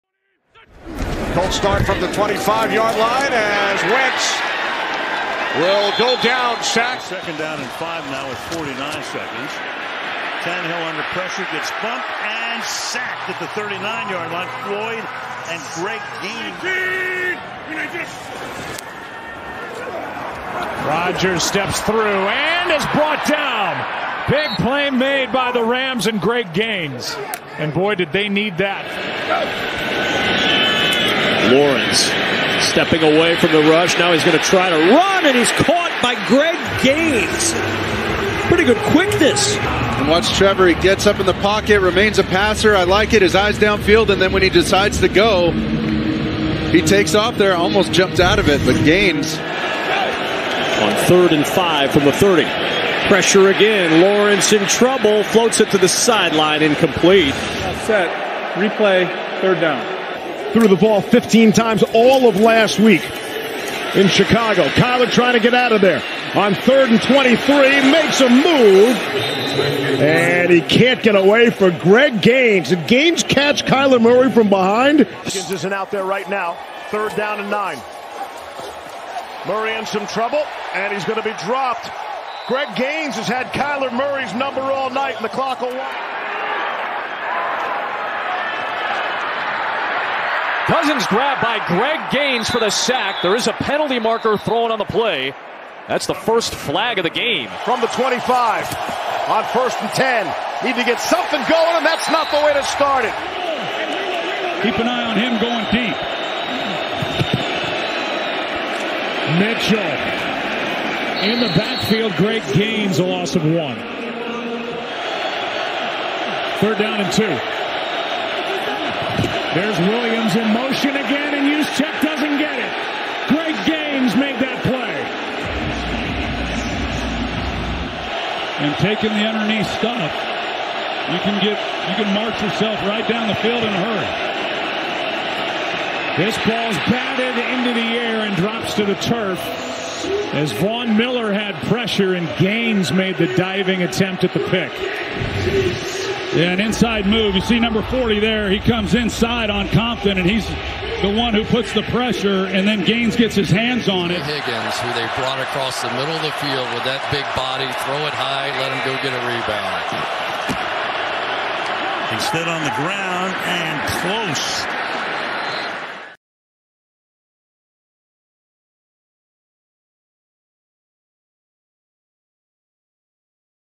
FULL Greg Gaines Highlights | The Cheetah | 2021-2022 NFL Season
Greg Gaines was drafted by the Los Angeles Rams in the fourth round of the 2019 NFL Draft out of Washington. Gaines was mostly a rotational player since his drafting, but in 2021, with the trade of Michael Brockers and the midseason injury of Sebastian Joseph-Day, Gaines secured a full time starting position. He became a star due to his solid play, his personality, and his ability to chase down quarterbacks with unusual speed.